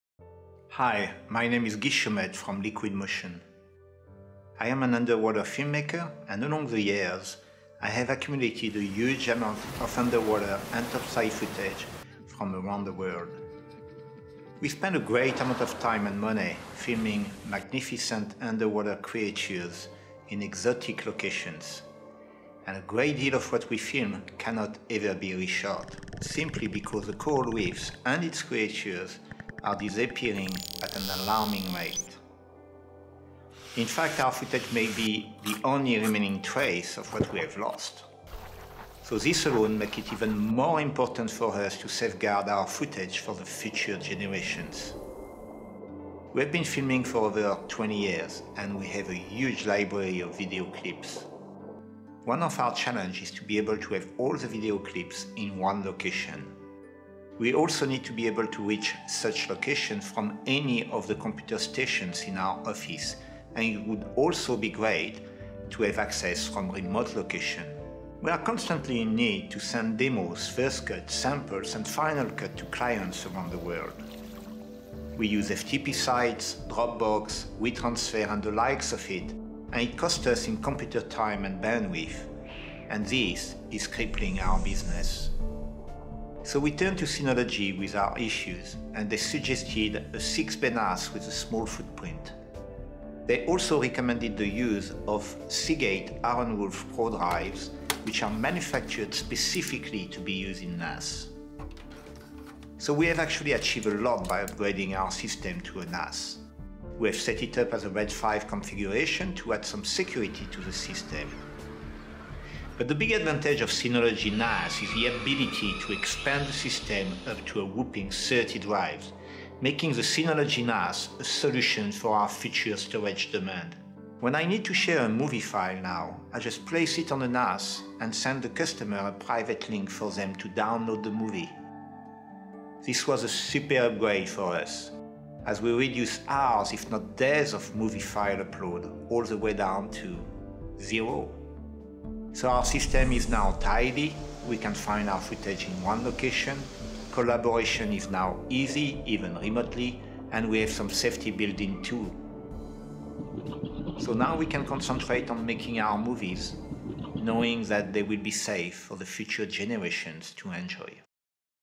Liquid Motion Film: Centralized Storage to Streamline Collaboration - Synology
Liquid Motion® Film is an internationally-acclaimed, multi-award-winning film production company. They are best known for their landmark underwater films which have appeared on the world’s leading TV channels.

Find out how Synology helped them streamline their workflow and protect their one-of-a-kind footage.